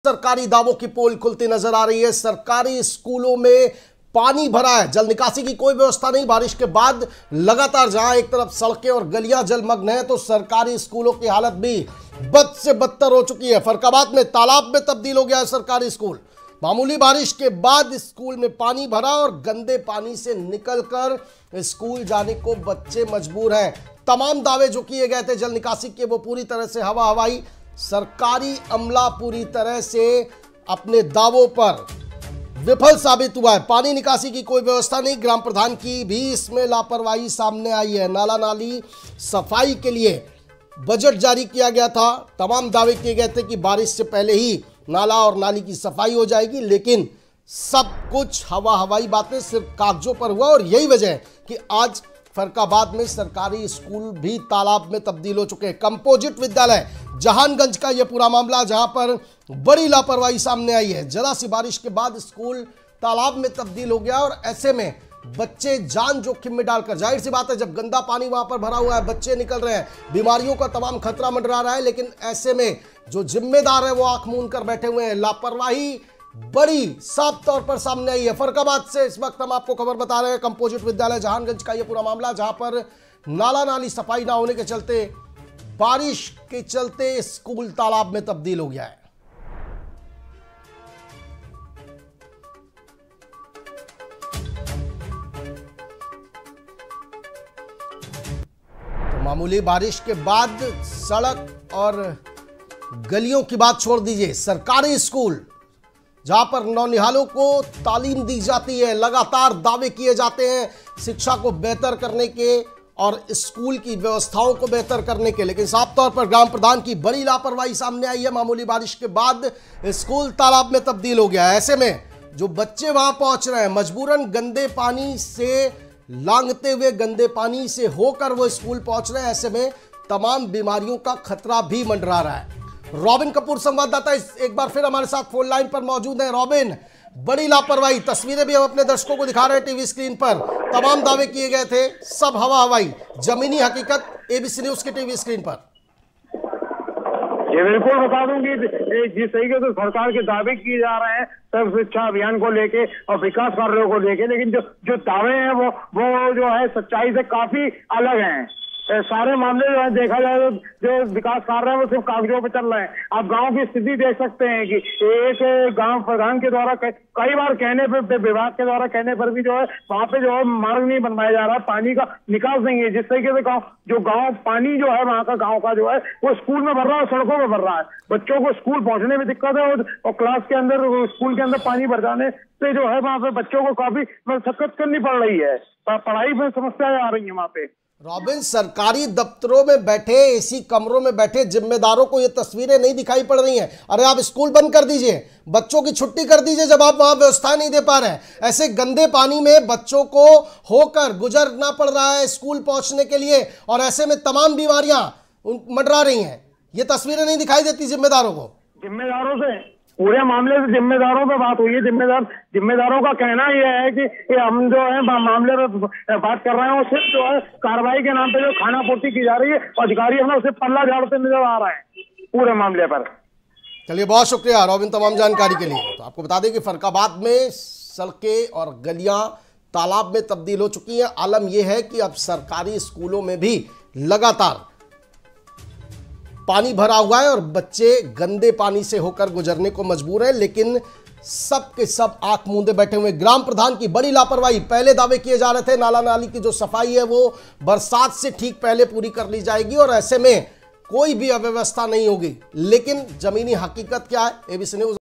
Farrukhabad - तालाब में तब्दील हुआ सरकारी स्कूल, जरा सी बारिश से स्कूल में भरा पानी | ABC NEWS
DEN- 157
SITI –85
JGD-99